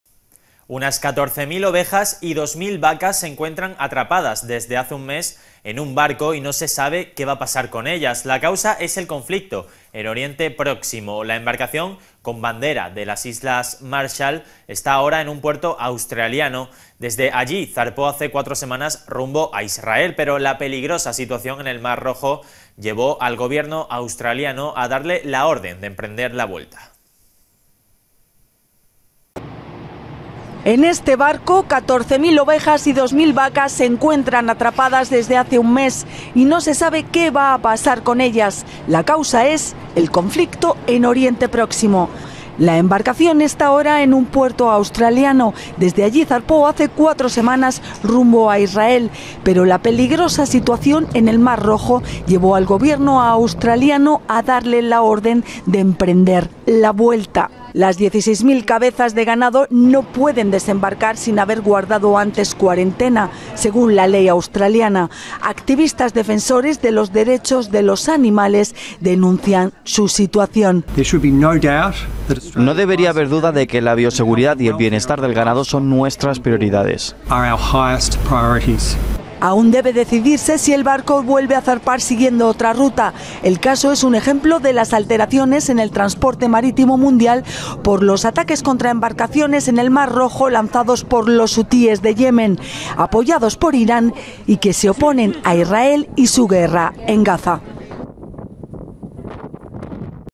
🔴Noticia - Reses varadas por el conflicto en el Mar Rojo
Unas 14.000 ovejas y 2.000 vacas se encuentran atrapadas desde hace un mes en un barco y no se sabe qué va a pasar con ellas. La causa es el conflicto en Oriente Próximo. La embarcación, con bandera de las Islas Marshall, está ahora en un puerto australiano. Desde allí zarpó hace cuatro semanas rumbo a Israel, pero la peligrosa situación en el mar Rojo llevó al Gobierno australiano a darle la orden de emprender la vuelta.
English:

Some 14,000 sheep and 2,000 cows have been trapped on a boat for a month and it is not known what will happen to them. The cause is the conflict in the Middle East. The ship, flying the flag of the Marshall Islands, is now in an Australian port. From there she set sail four weeks ago for Israel, but the dangerous situation in the Red Sea led the Australian Government to order her to return.

Live streaming